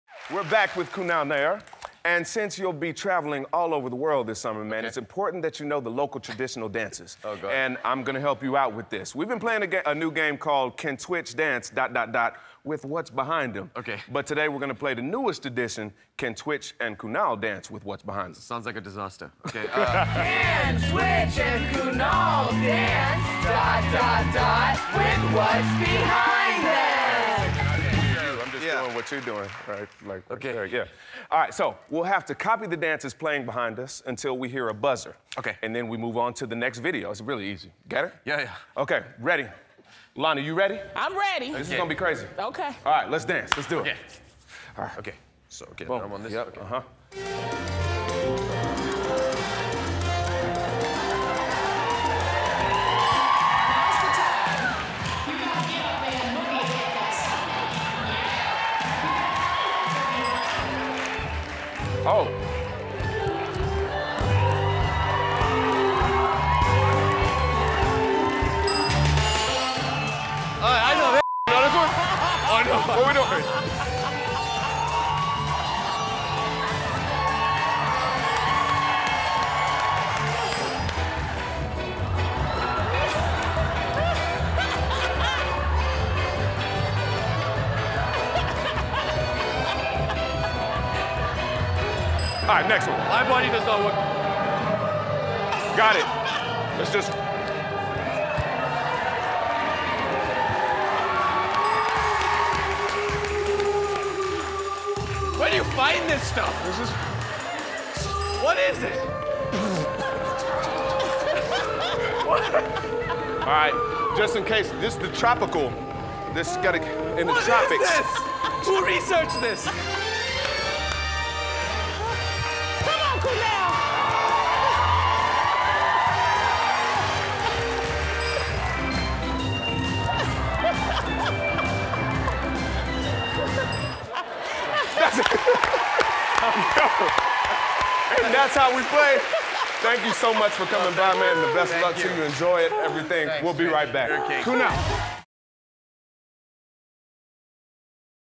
Kunal Nayyar and tWitch Play 'Can Twitch and Kunal Dance... with What’s Behind Them?
Kunal Nayyar has a bunch of international travel goals this summer, so to help prepare "The Big Bang Theory" star for encountering different cultures, guest host tWitch tested his dancing skills in a game called, "Can tWitch and Kunal dance... with What's Behind Them?"